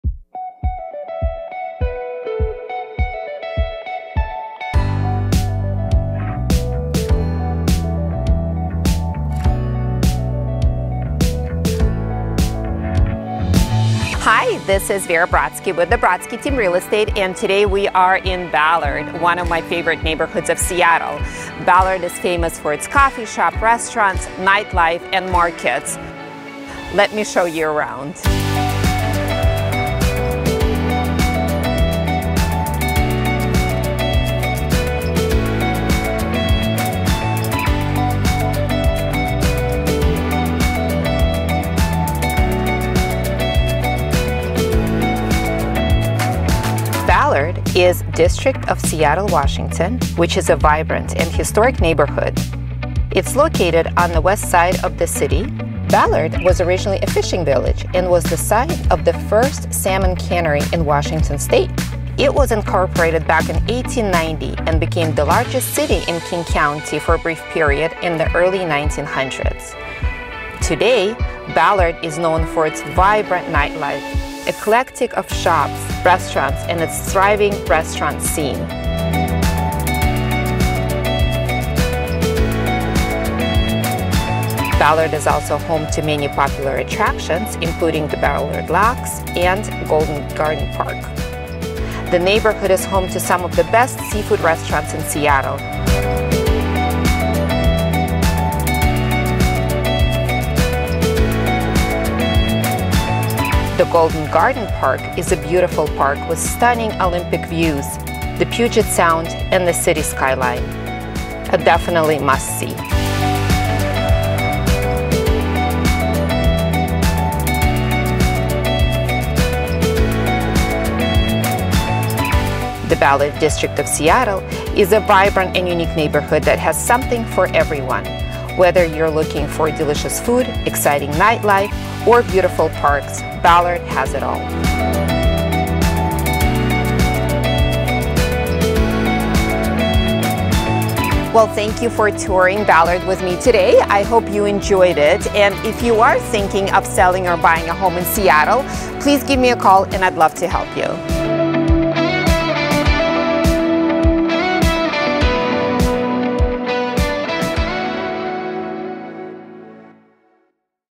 Ballard, Seattle | Neighborhood Tour with Vera Brodsky
Welcome to Ballard!

Let’s discover what it would be like to live in Ballard, a beautiful neighborhood in Seattle, Washington! 🏙️

Ballard is a lively neighborhood located on the waterfront, which boasts a range of trendy restaurants, independent shops, bars, and craft breweries. The area is centered around the historic Ballard Avenue and is known for its vibrant atmosphere. One of the main attractions in the neighborhood is the Hiram M. Chittenden Locks, also known as the Ballard Locks, which is a popular spot to watch ships and salmon pass through. It's also a great place for a picnic with friends or family. Another popular destination in Ballard is Golden Gardens Park, which features a sandy beach that is perfect for sunbathing and volleyball. The park also offers breathtaking views of the nearby mountains.

I would love to help you find your new house.